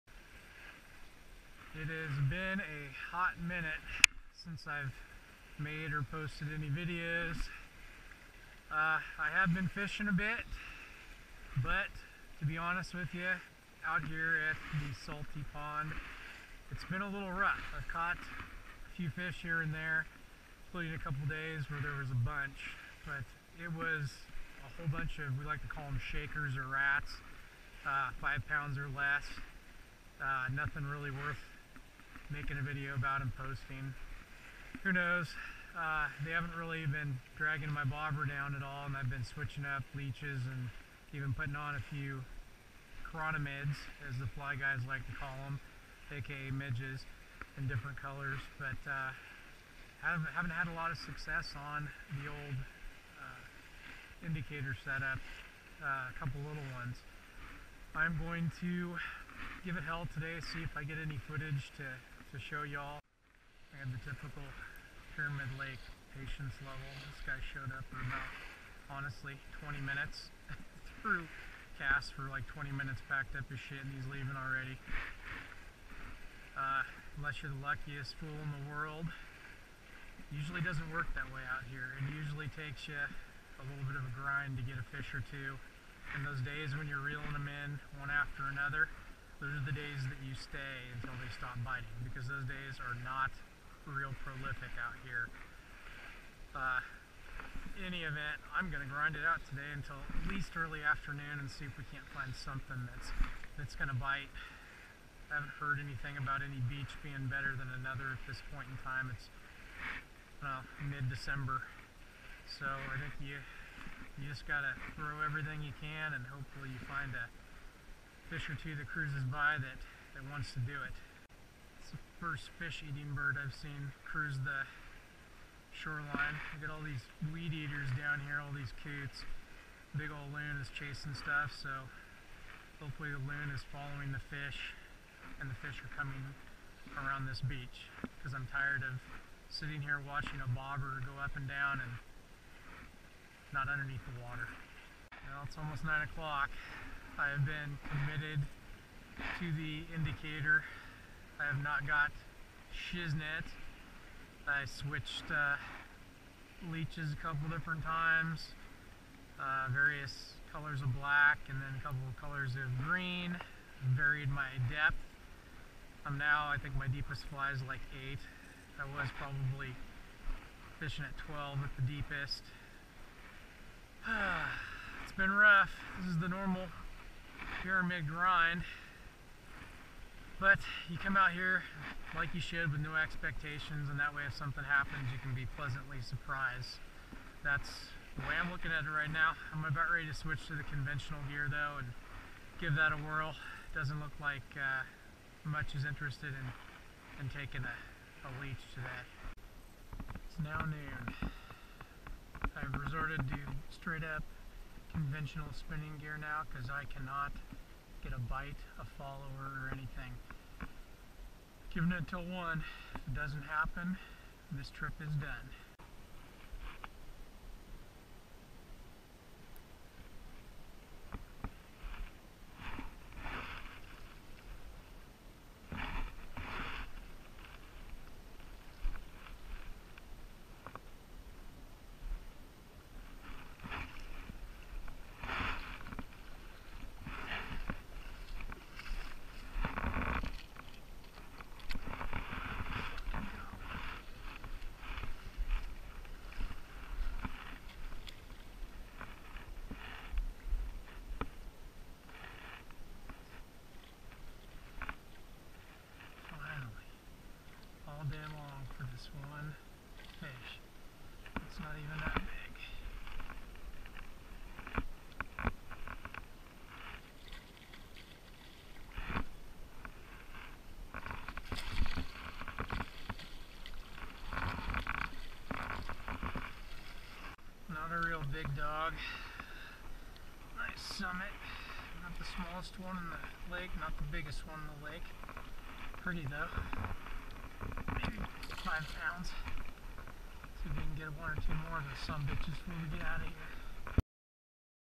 Mid-December Doldrums at The Salty Pond - A Classic Example of a Pyramid Lake GRIND
Not the most exciting fishing video I've created but if I didn't post some of this trip then I'd go yet another week without a post.  With that being said, this shows how tough it can be certain times of year (like Decembers) to get any fish to bite at Pyramid Lake, let alone one of the big ones we all go out there to chase.  I'm pretty sure the weather isn't what's making these fish lazy and not wanting to chase or eat and, instead, it has something to do with the recent overall drop in water temperature...the fish just aren't used to it yet.  Anyhow, threw everything I had today and only ended up with one mediocre/average size Summit.  Things gotta pick up out there soon...OH, the eternal optimism (or ignorance) of a fisherman!